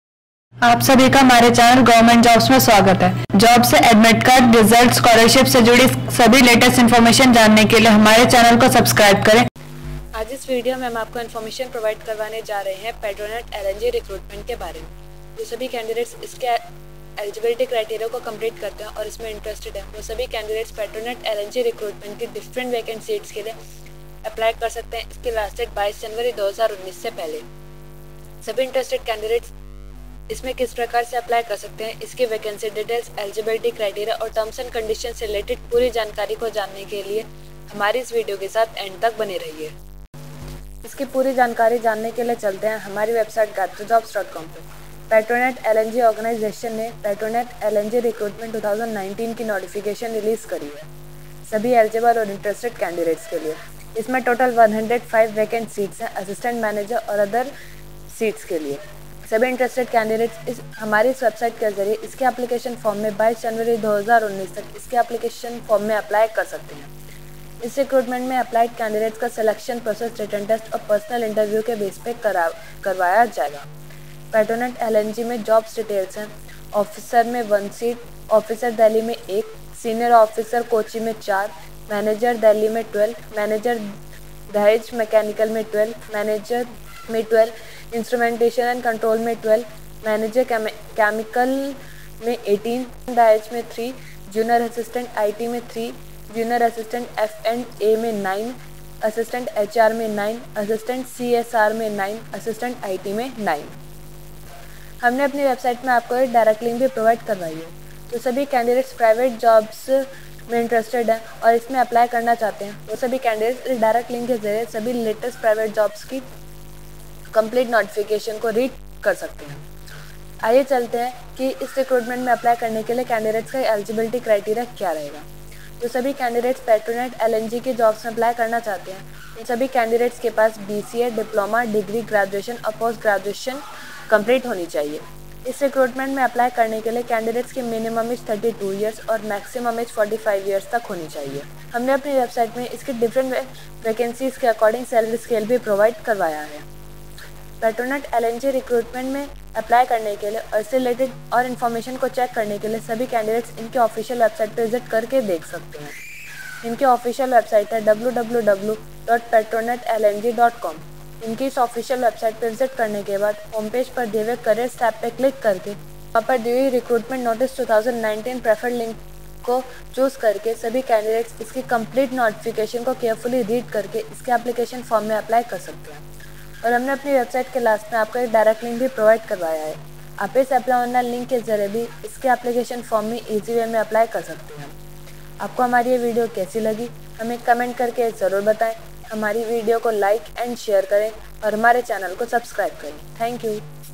PETRONET LNG Recruitment 2019 for various Vacancies
The PETRONET LNG Recruitment 2019 Notification for filling up 105 posts for the Assistant Manager & Others has issued, Apply Online PETRONET LNG Current Vacancies @